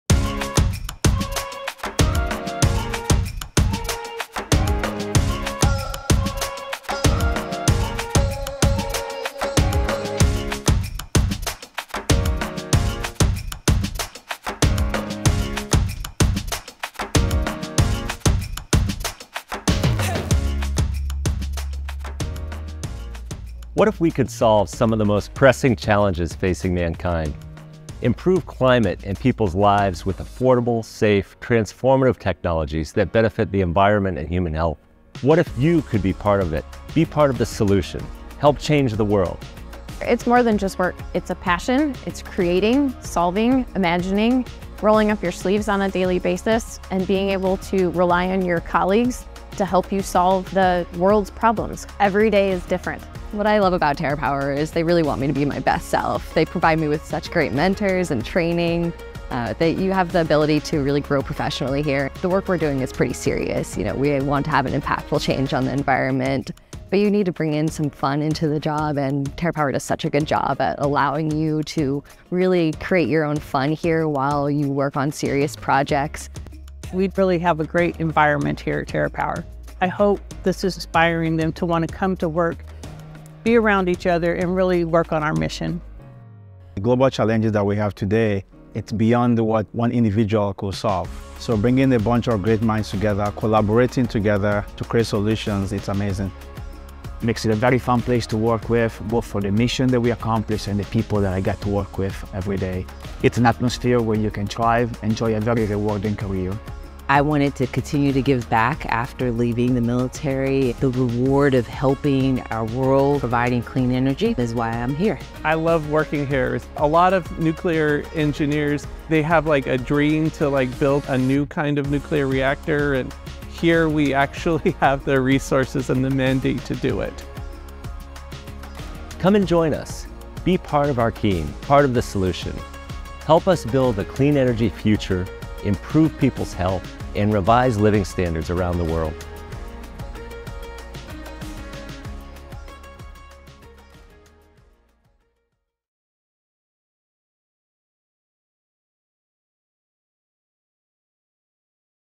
Why should you consider working at TerraPower?
At TerraPower, it's not only about battling climate change. Our clean energy solutions will lift billions out of poverty, advance cancer treatments, and provide safe, clean energy around the globe. Join us, and be part of the next generation of talented innovators.